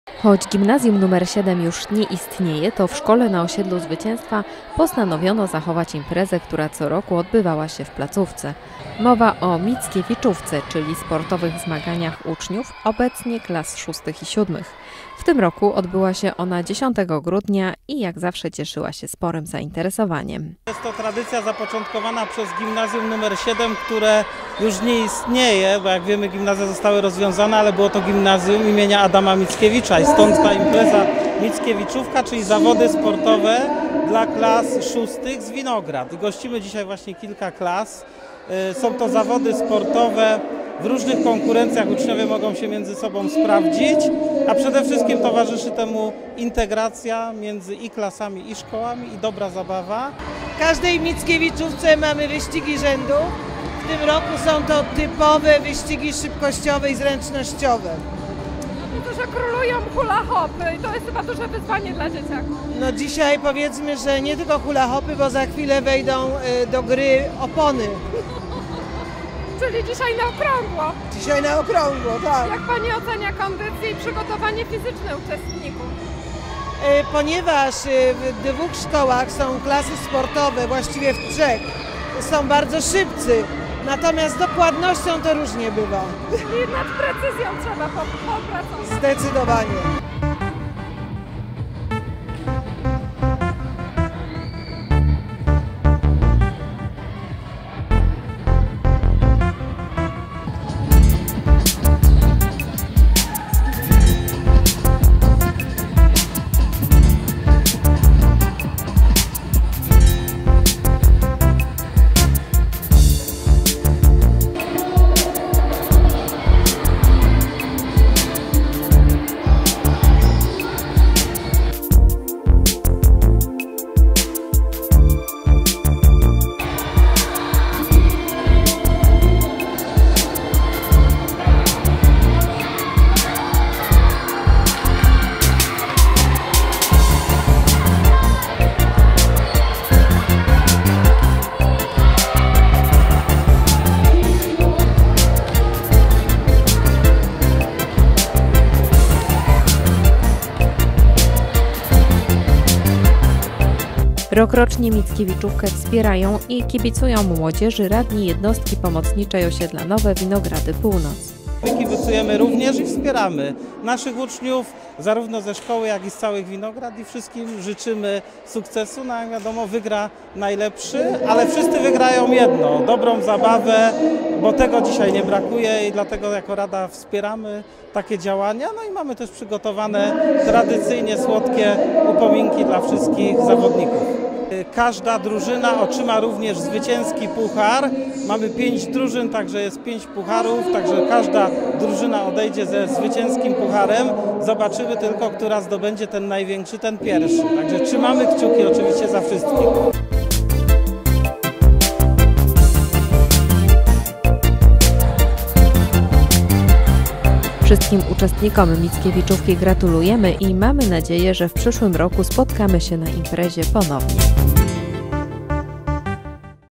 2019.12.10 Mickiewiczówka 2019 w Zespole Szkół nr 7
We wtorek 10 grudnia w Zespole Szkół nr 7 na Osiedlu Zwycięstwa po raz dwudziesty odbyły się zawody sportowe MICKIEWICZÓWKA 2019 🏃‍♀️🏃‍♂️🤸‍♀️🤸‍♂️⛹️‍♀️⛹️‍♂️🏆📜🎁 dla klas IV, V, VI, VII i VIII szkół podstawowych z Winograd.

Organizatorem zawodów byli: Zespół Szkół nr 7 w Poznaniu i Rada Osiedla Nowe Winogrady Północ.

Puchary dla drużyn i słodkie niespodzianki dla wszystkich zawodników ufundowała Rada Osiedla NOWE WINOGRADY PÓŁNOC – Jednostka Pomocnicza Miasta Poznania.

Dziękujemy za świetną zabawę.

Materiał filmowy został zrealizowany przez Telewizję WTvK.